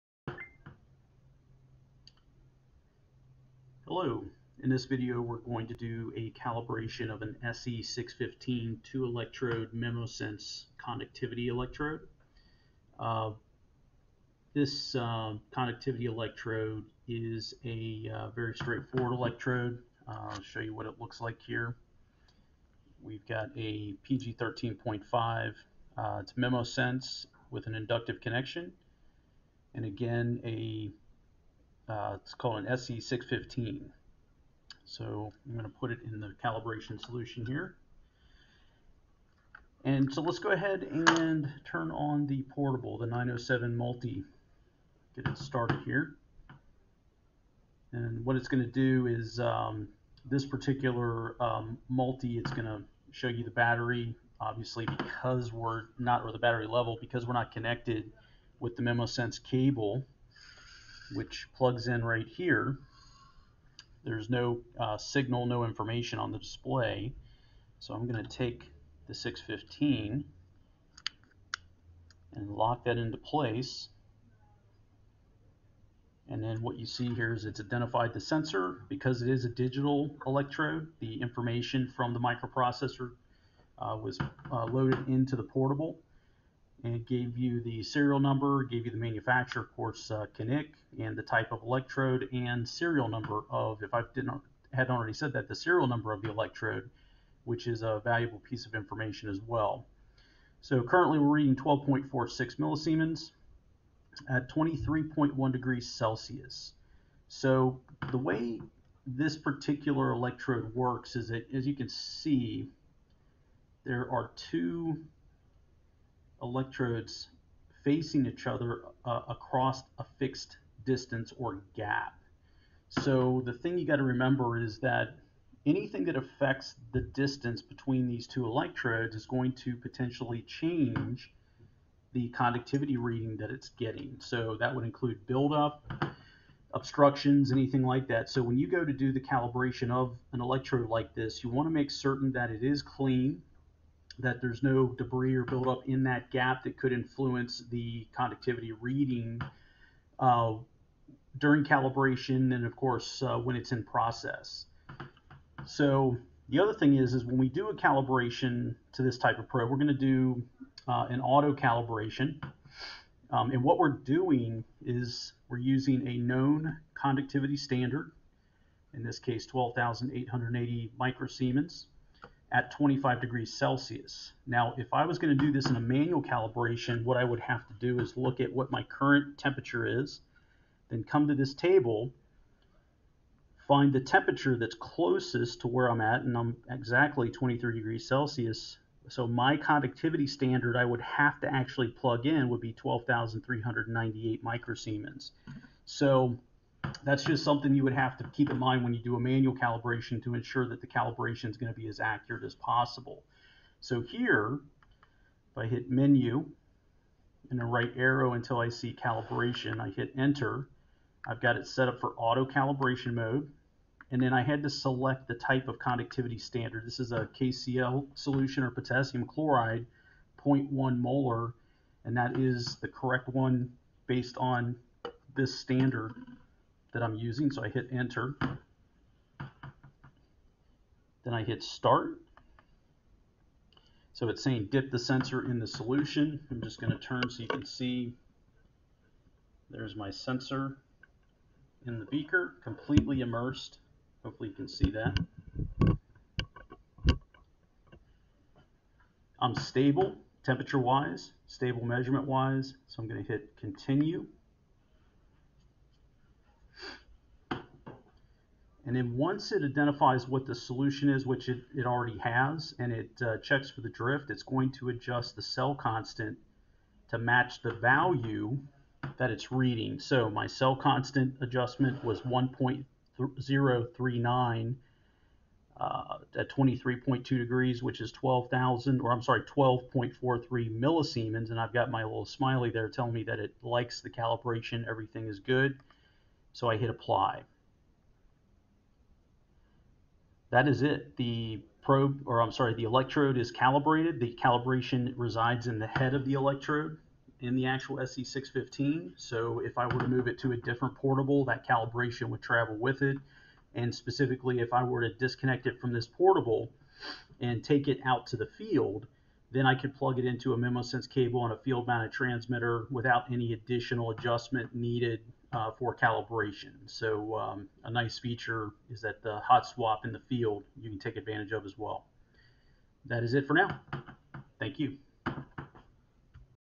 SE 615 Conductivity Sensor Automatic Calibration with Portable Meter
Hello, in this video we're going to do a calibration of an SE 615 2 electrode Memosens conductivity electrode.

This conductivity electrode is a very straightforward electrode, I'll show you what it looks like here.

We've got a PG 13.5 - it's Memosens with an inductive connection and again it's called an SE 615.

So, I'm going to put it in the calibration solution here, and so let's go ahead and turn on the portable, the 907 Multi.

Get it started here, and what this particular Multi is going to show you the battery level.

Obviously because we're not connected with the Memosens cable which plugs in right here, there's no signal - no information on the display.

So I'm going to take the 615, and lock that into place, and then what you see here is it's identified the sensor.

Because it is a digital electrode, the information from the microprocessor was loaded into the portable and gave you the serial number, gave you the manufacturer - of course Knick - and the type of electrode.

So, currently we're reading 12.46 millisiemens at 23.1 °C.

So, the way this particular electrode works is that as you can see there are 2 electrodes facing each other across a fixed distance or gap.

So the thing you got to remember is that anything that affects the distance between these 2 electrodes is going to potentially change the conductivity reading that it's getting.

So that would include build up, obstructions, anything like that.

So, when you go to do the calibration of an electrode like this you want to make certain that it is clean, that there's no debris or buildup in that gap that could influence the conductivity reading during calibration, and of course when it's in process.

The other thing is is when we do a calibration to this type of probe, we're going to do an auto calibration.

And what we're doing is we're using a known conductivity standard, in this case 12880 microsiemens at 25 °C.

Now, if I was going to do this in a manual calibration, what I would have to do is look at what my current temperature is, then come to this table, find the temperature that's closest to where I'm at - and I'm exactly 23 °C so my conductivity standard I would have to actually plug in would be 12398 microsiemens.

So, that's just something you would have to keep in mind when you do a manual calibration to ensure that the calibration is going to be as accurate as possible.

So here if I hit Menu, and the right arrow until I see Calibration, I hit Enter; I've got it set up for Auto calibration mode, and then I had to select the type of conductivity standard.

This is a KCL solution or potassium chloride 0.1 molar, and that is the correct one based on this standard that I'm using, so I hit Enter, then I hit Start.

So it's saying dip the sensor in the solution, I'm just going to turn so you can see, there's my sensor in the beaker completely immersed, hopefully you can see that.

I'm stable temperature-wise, stable measurement-wise so I'm going to hit Continue, and then once it identifies what the solution is and it checks for the drift, it's going to adjust the cell constant to match the value that it's reading.

So, my cell constant adjustment was 1.039 at 23.2 °C which is 12.43 millisiemens, and I've got my little smiley there telling me that it likes the calibration everything is good, so I hit Apply.

That is it, the electrode is calibrated.

The calibration resides in the head of the electrode in the actual SE 615.

So, if I were to move it to a different portable that calibration would travel with it, and specifically if I were to disconnect it from this portable and take it out to the field, then I could plug it into a Memosens cable on a field mounted transmitter without any additional adjustment needed for calibration.

So, a nice feature is the hot swap in the field you can take advantage of as well.

That is it for now, thank you.